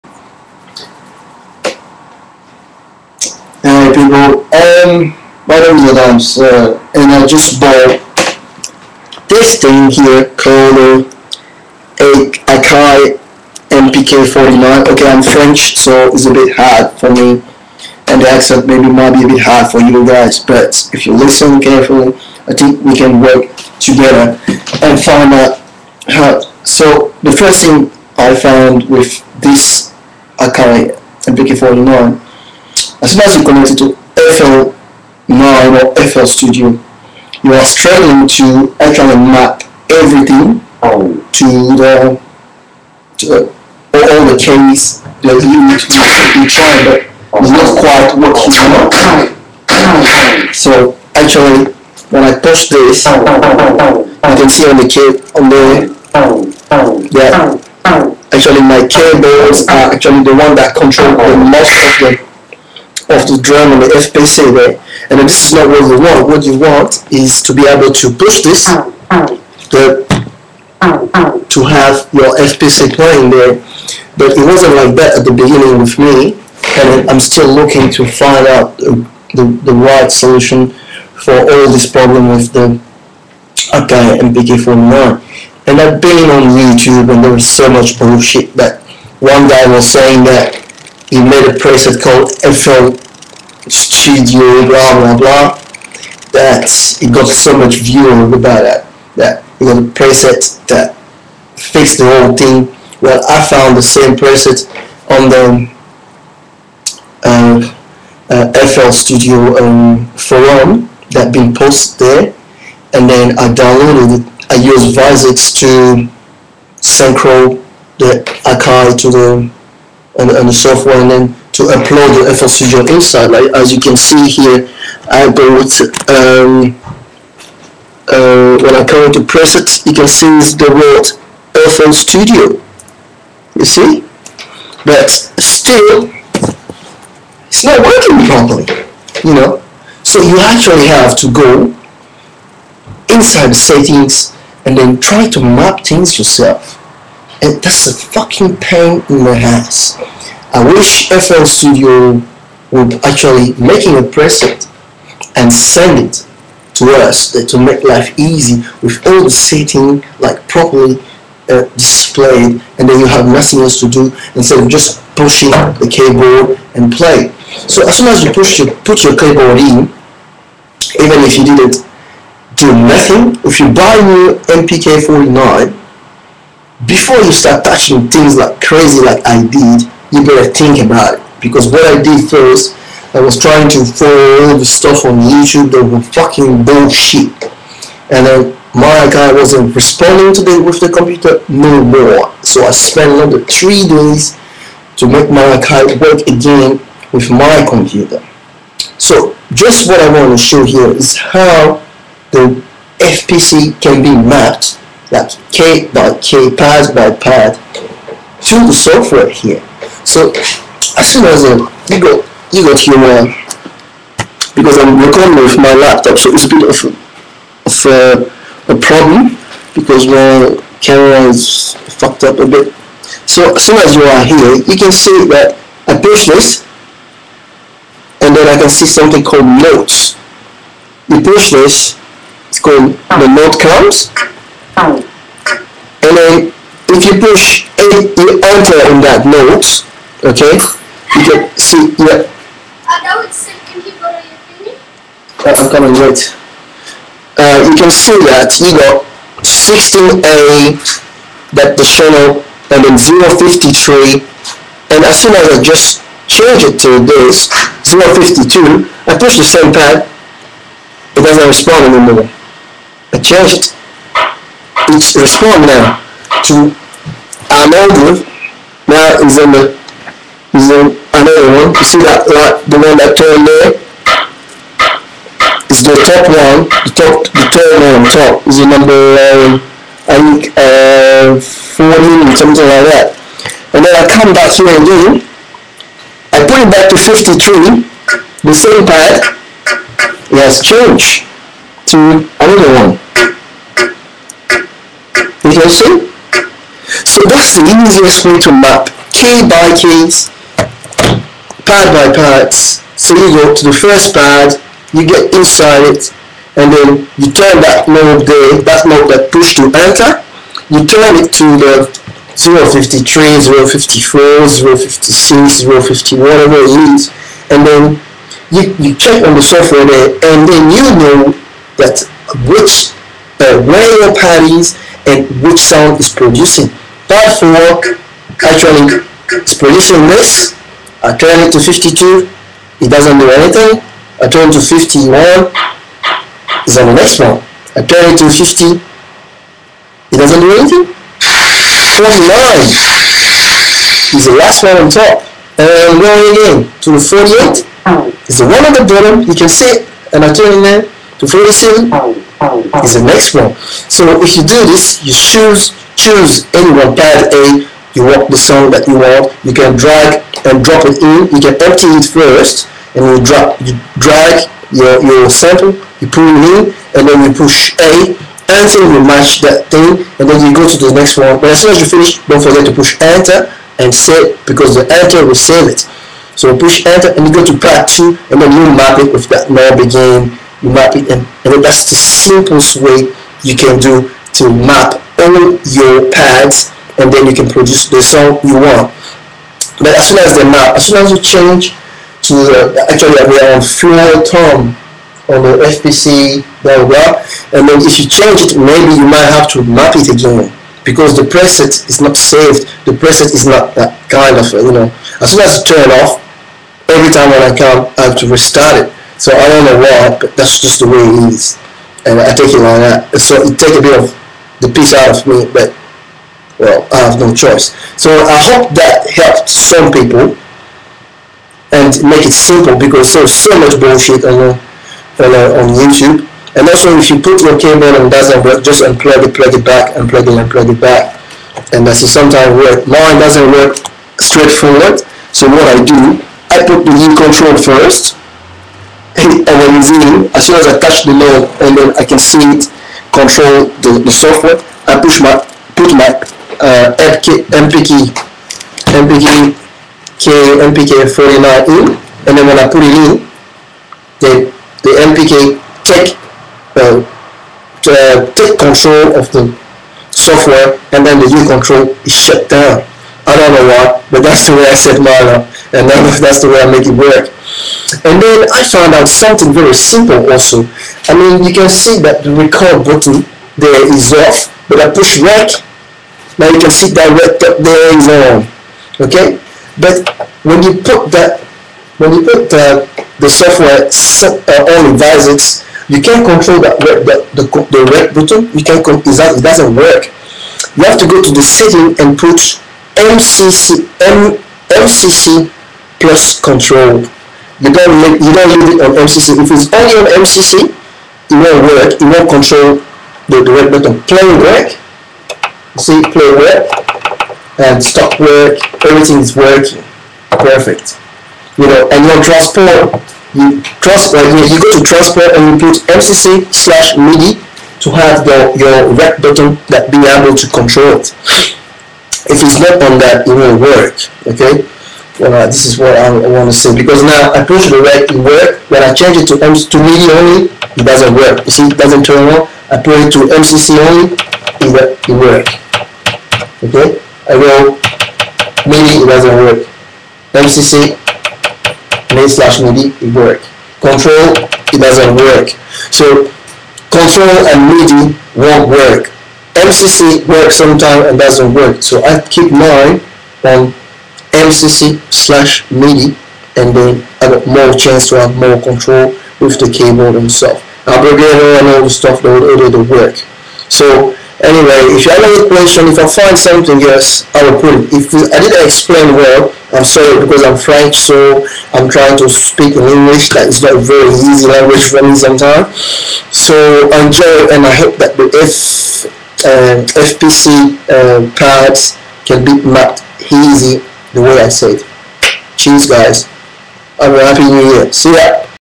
Akai MPK49 and FLstudio mapping
how can you map the hardware pads to the digitals pads in fl9